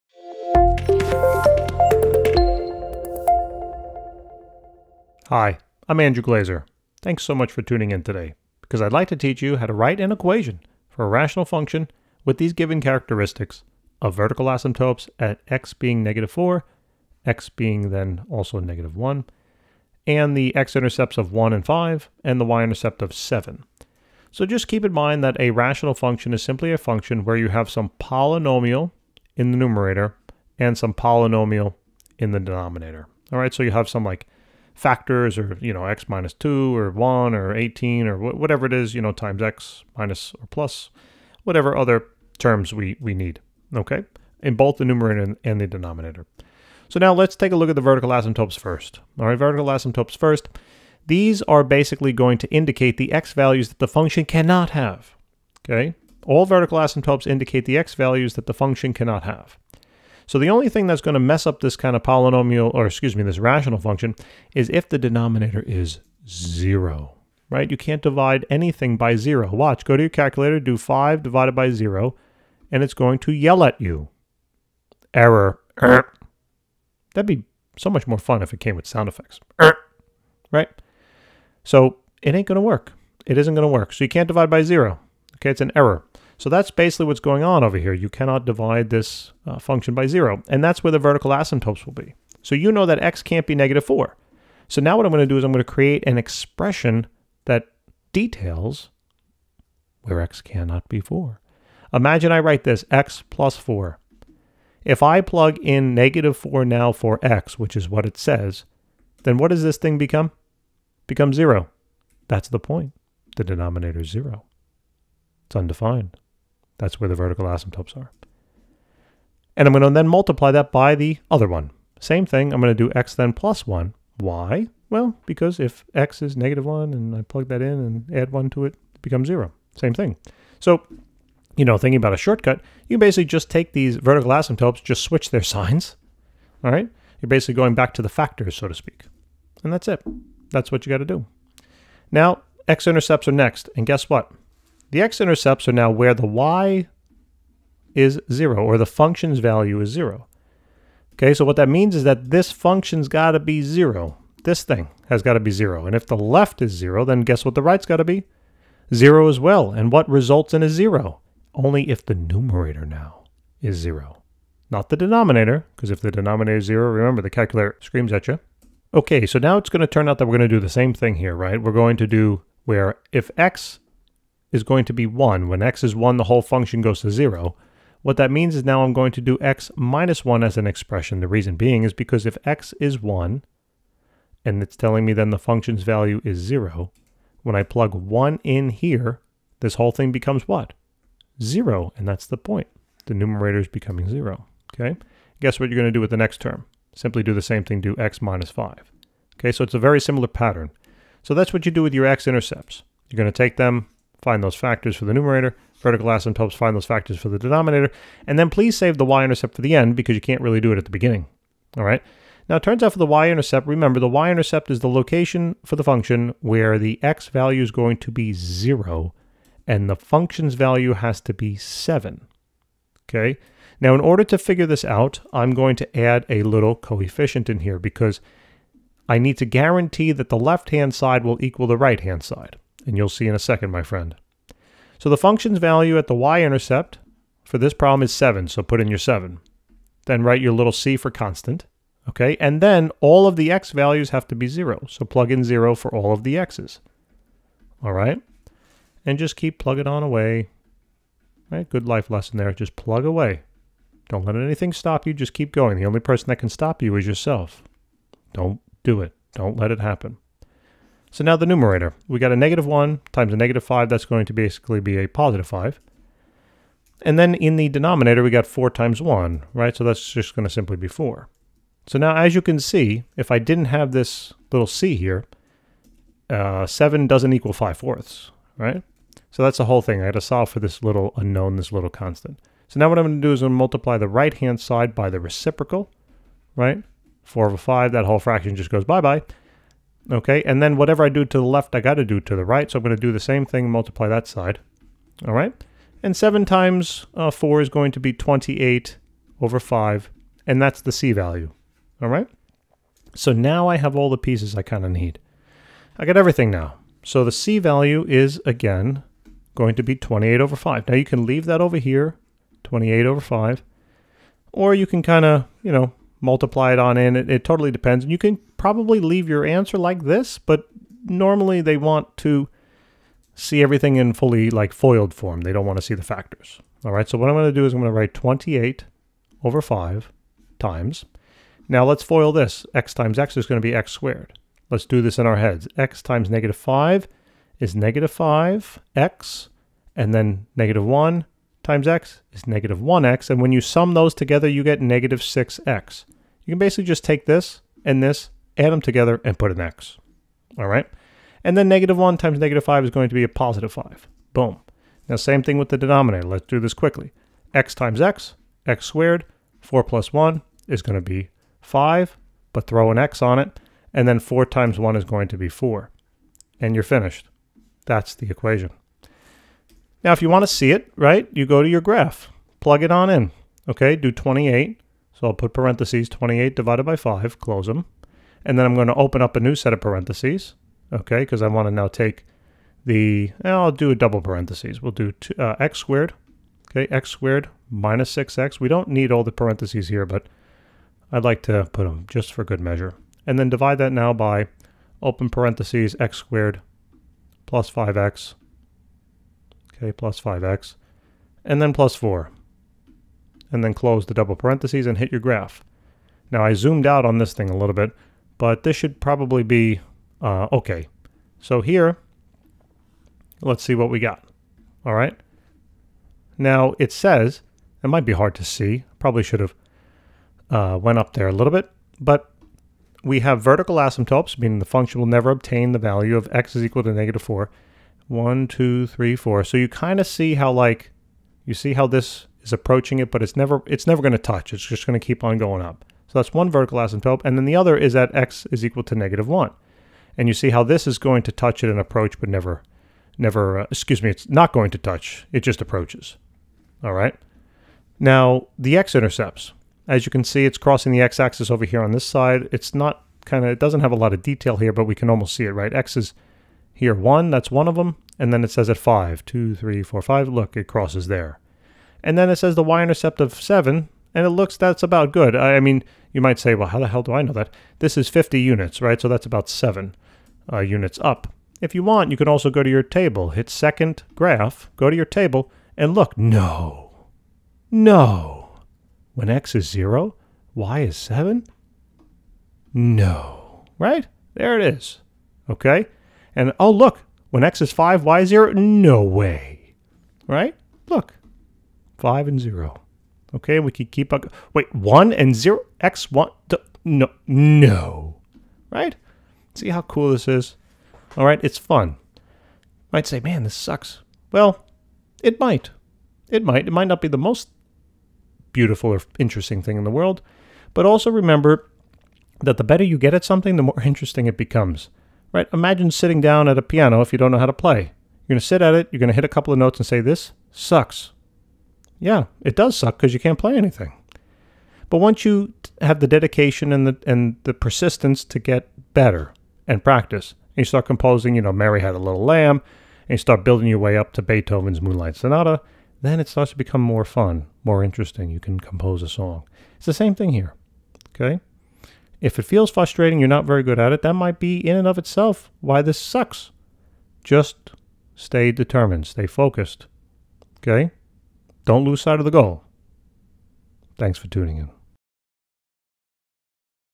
How to write an equation with vertical asymptotes at x = −4 and x = −1, x-intercepts at (1, 0) and
For the following exercises, write an equation for a rational function with the given characteristics.
Vertical asymptotes at x = −4 and x = −1, x-intercepts at (1, 0) and (5, 0), y-intercept at (0, 7)